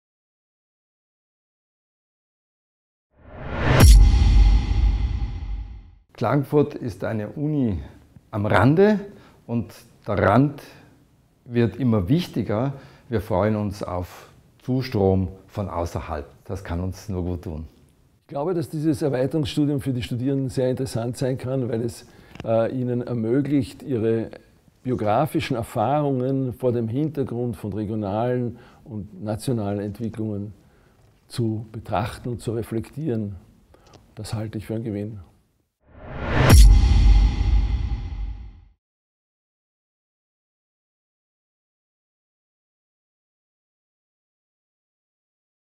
Erweiterungsstudium Inter-/Transkulturelle Kompetenzen – Was Lehrende noch sagen möchten.
Hans Karl Peterlini und Georg Gombos, Lehrende im Erweiterungsstudium Inter-/Transkulturelle Kompetenzen abseits der gestellten Fragen.